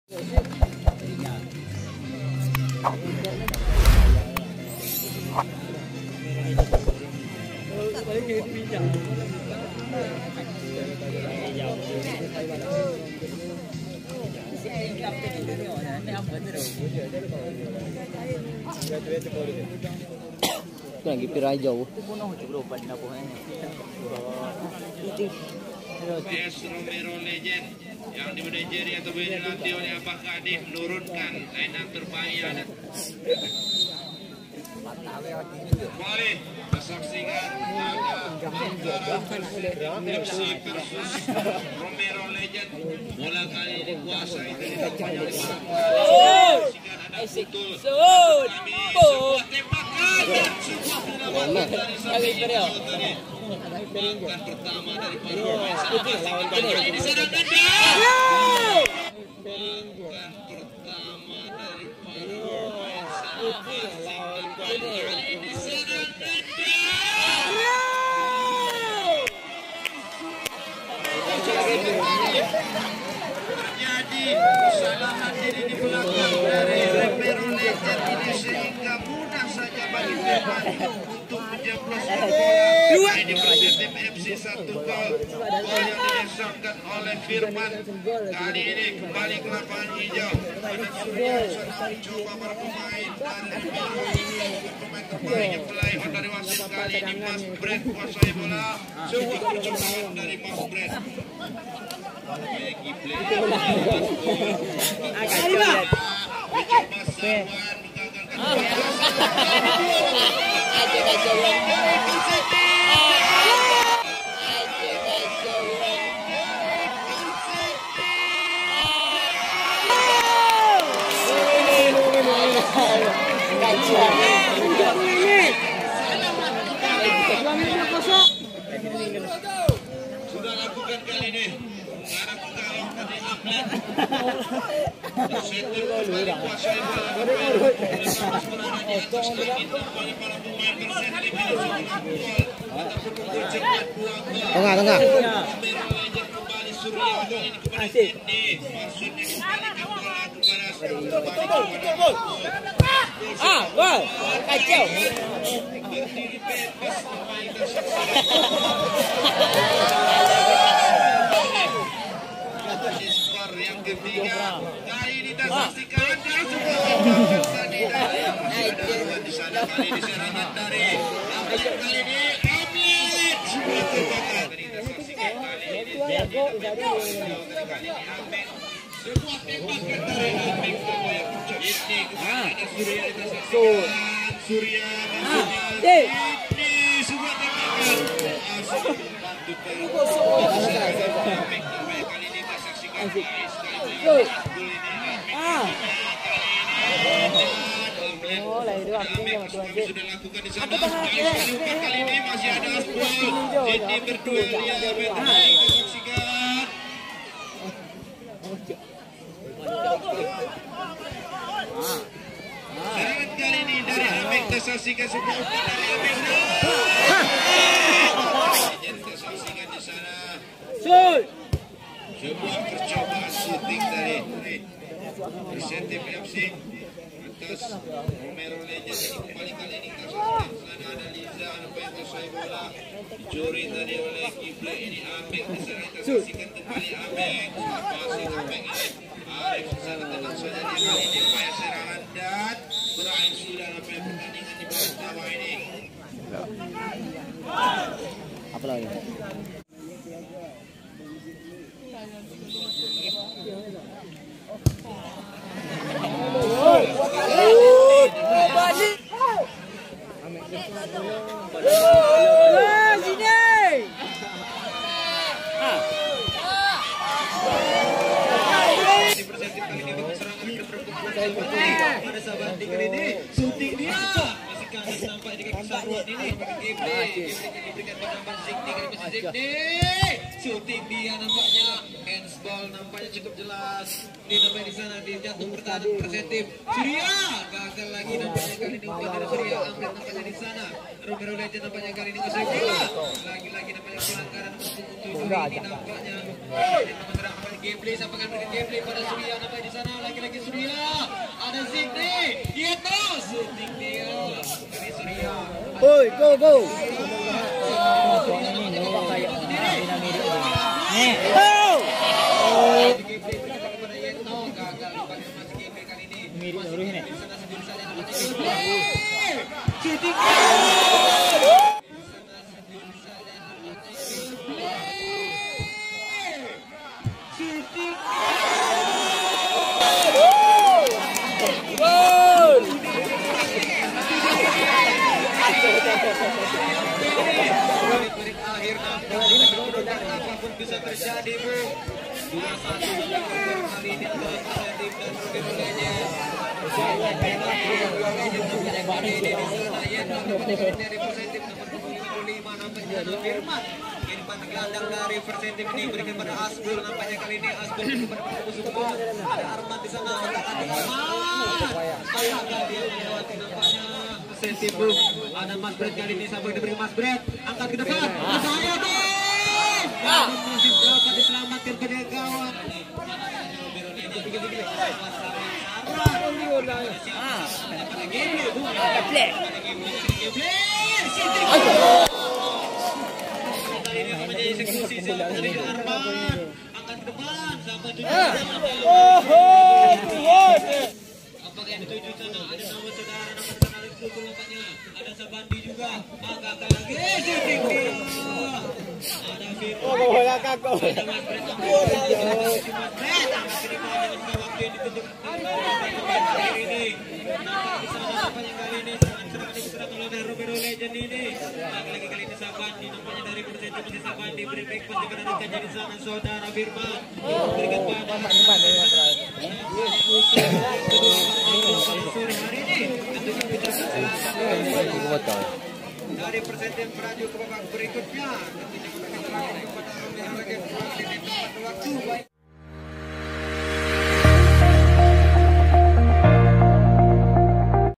ROMERO LEGEND vs PERSETIM FC | Highlight MINI SOCCER ARBET CUP V
Open Turnamen mini soccer ARBET CUP V DUSUN AUR BETUNG TAHUN 2023